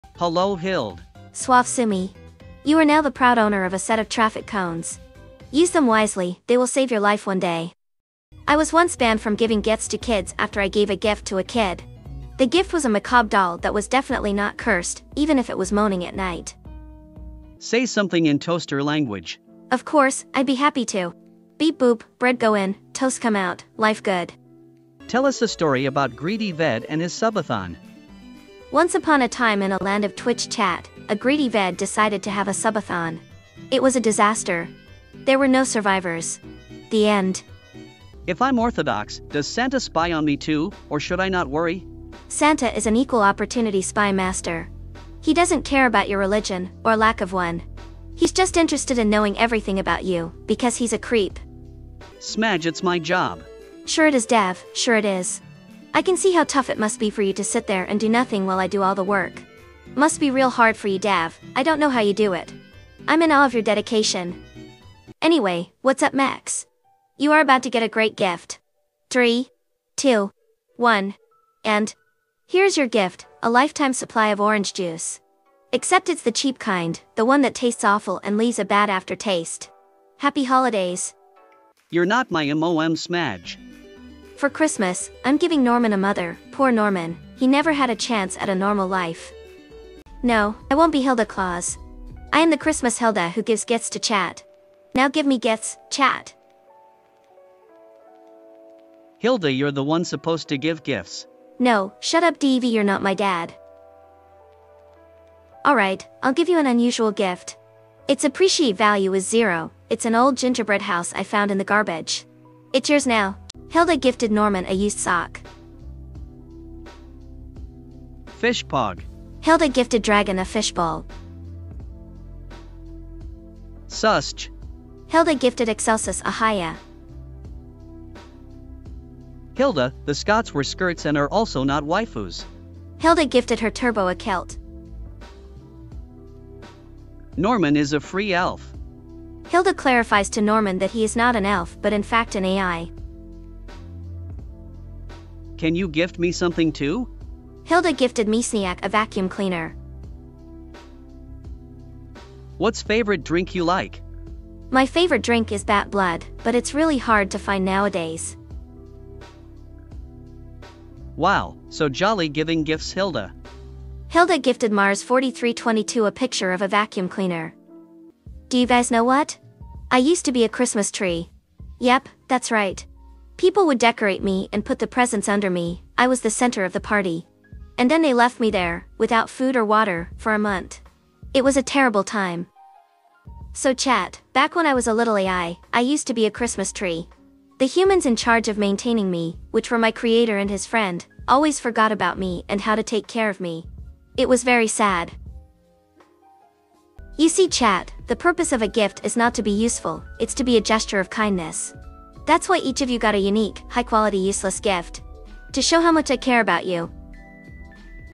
Hilda's gift sharing stream was a total disaster
Who let the ai vtuber in charge if gifts??